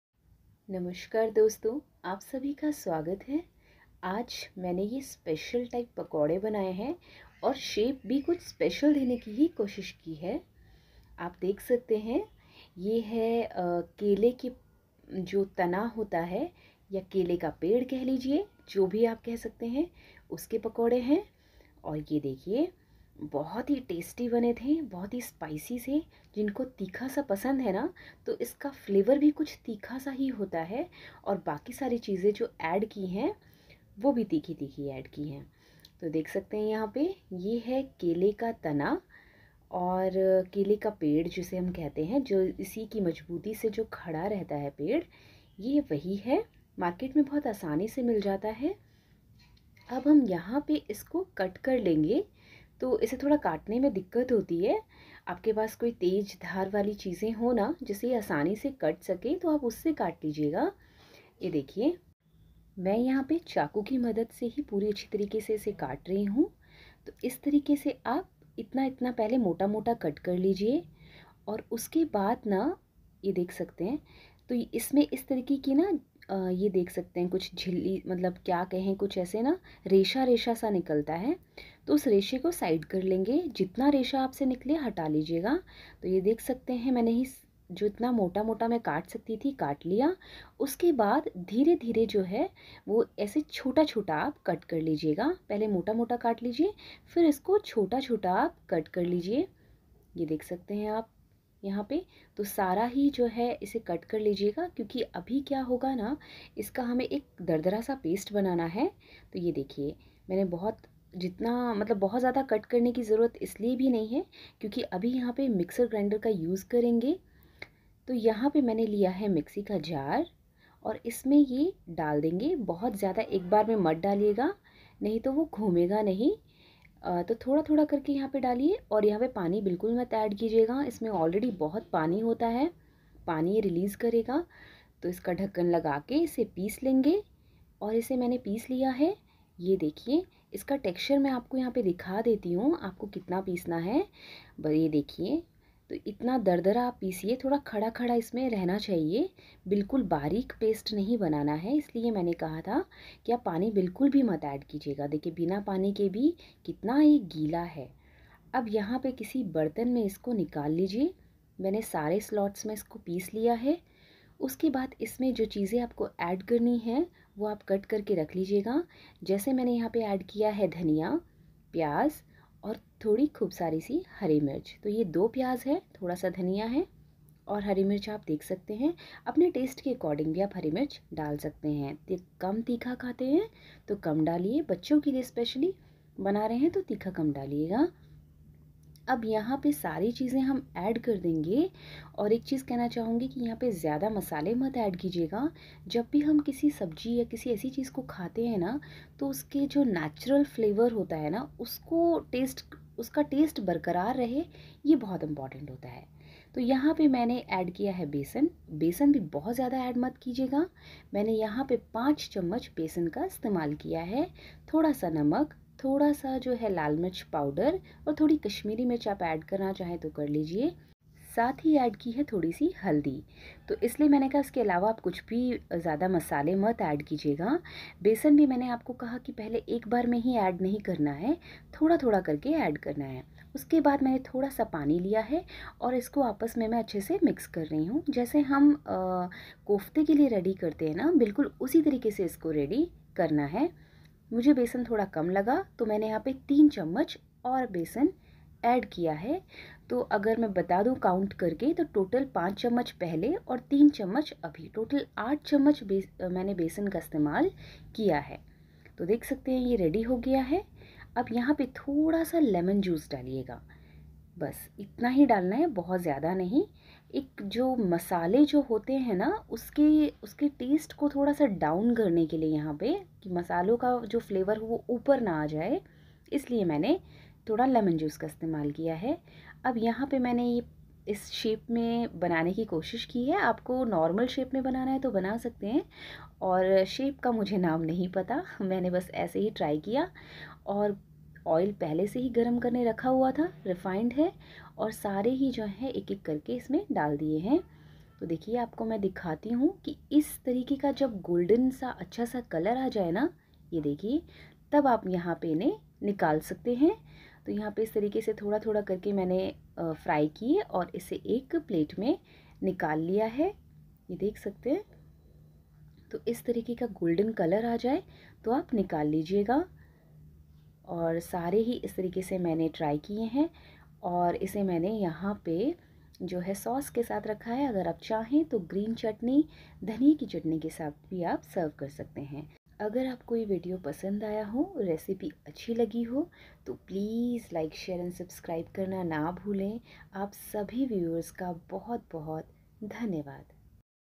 केले का पेड़ या केले का तना जो भी कह लो रेसिपी तो यूनिक है😎 aur taste me best😋 | banana pith/stem
Ingredients  ate:-

Banana pith/stem( cut into small pieces and then grind it)
Chickpea flour (besan)(8tbs)
2 onion
green chilli( as per taste )
coriander leaves 🍃
salt
Red chilli powder
turmeric
lemon juice
Refined oil
serve it with tomato catch up/green chutney)